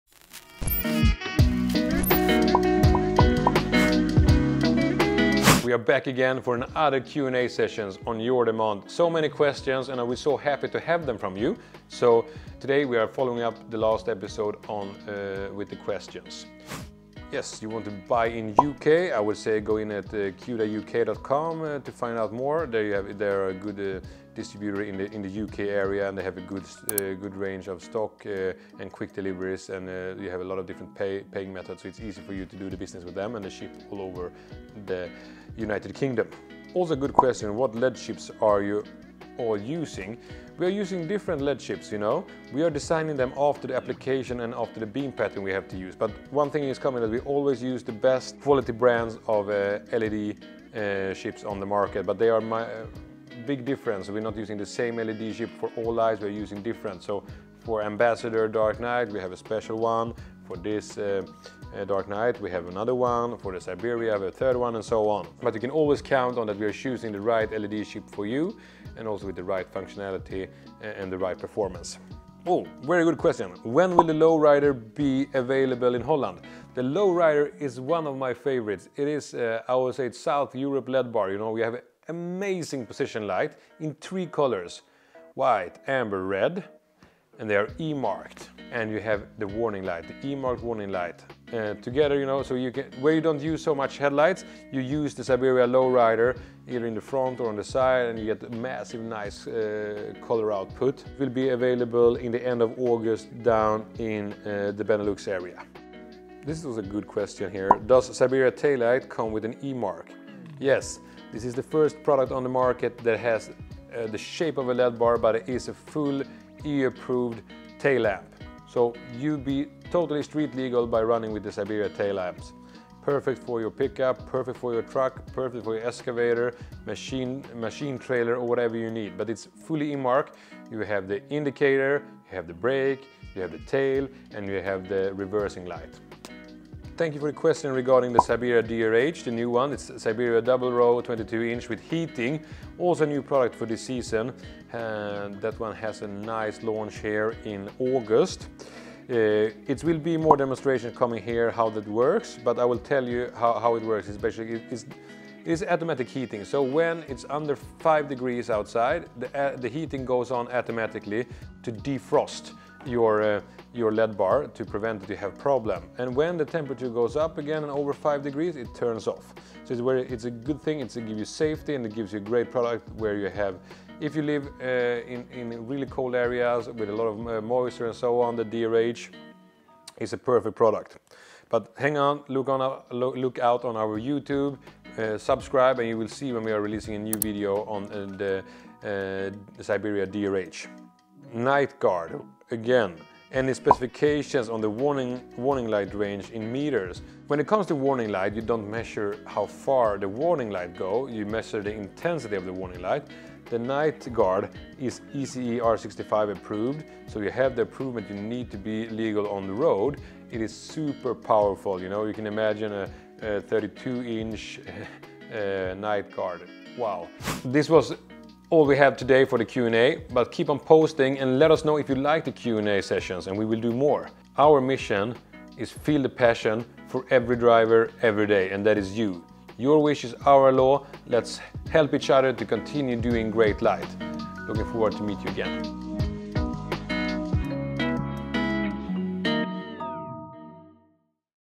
CHECK OUT OUR FIRST Q/A – PART 3 – STRANDS LIGHTING DIVISION
Our CEO Johan Carlos answers the questions you have been asking us on our Instagram stories (@strandslightingdivision). Please comment if you have more questions, and we can bring them up in future Q&As. We are looking forward to your questions!

🔥 LINKS TO MENTIONED PRODUCTS...

SIBERIA DRH
→ Coming in August 2022

See you in the following video!